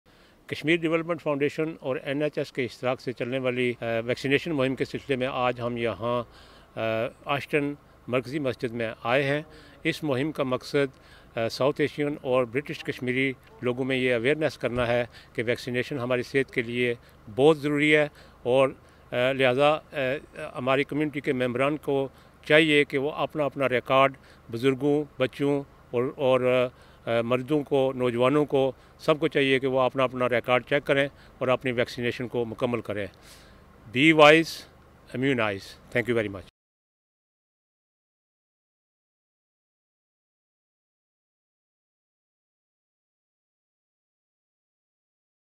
KDF Vaccine Literacy Campaign, Ashton, Husain urd
Vaccines are our best protection.
ویکسین اساں نی حفاظت ناں بہترین ذریعہ ہین۔
ویکسین ہماری حفاطت کا بہترین ذریعہ ہیں
If you have missed a vaccine, contact your GP practice to catch up.

Last winter, 190,000 people in England were hospitalised due to COVID-19 or flu.
Millions of people are being offered vaccination to help protect them over the coming months.
Who can get the Vaccination?
You can get free flu and COVID-19 vaccines this winter if you:
• are 65 or over.
• have a certain health condition.
• are pregnant.
• are a frontline health or care professional.
• are an unpaid carer.
• live with someone who has a weakened immune system.
Children aged 2 or 3 years on 31 August 2023 and school aged children (from Reception to Year 11) can also get the flu vaccine.
Be wise, Immunise.
This video was produced by KDF for the provision of engagement and communication activities as part of Vaccine literacy campaign to increase COVID-19 and other routine vaccination uptake within British Kashmiri and South Asian communities. Project Number: 1563 commissioned by NHSE.